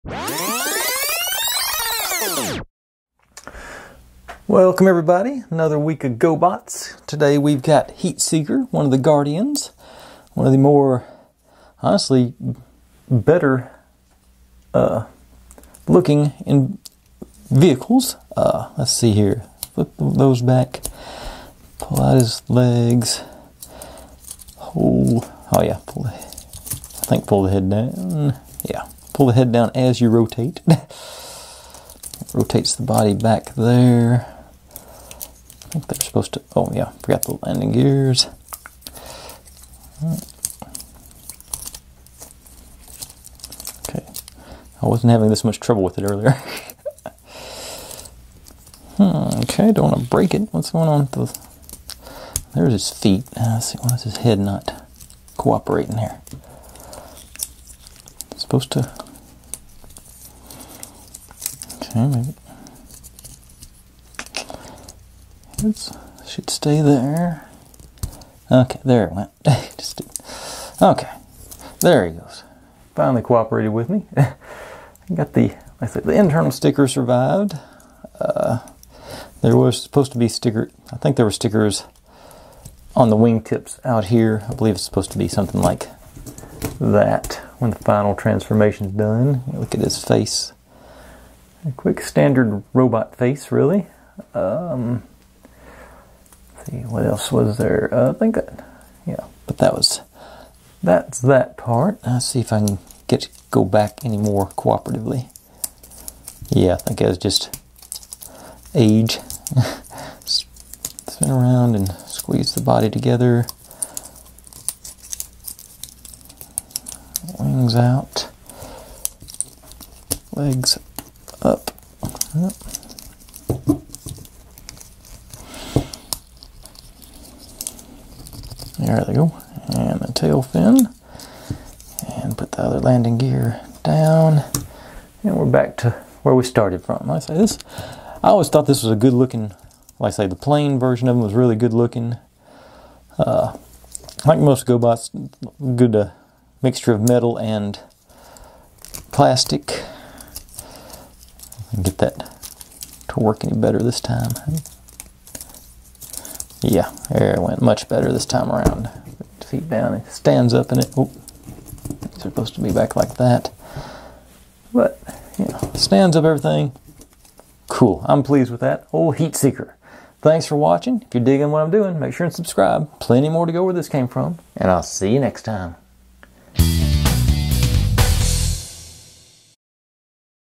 Checking Out My Old Toys:Go-Bots: Heat Seeker
My parents finally forced me to get my old toys out of their basement, so I'm going to go through them here on YouTube, I'm looking forward to trying to remember them. I plan to try and give them a gentle cleaning and store and organize them properly now.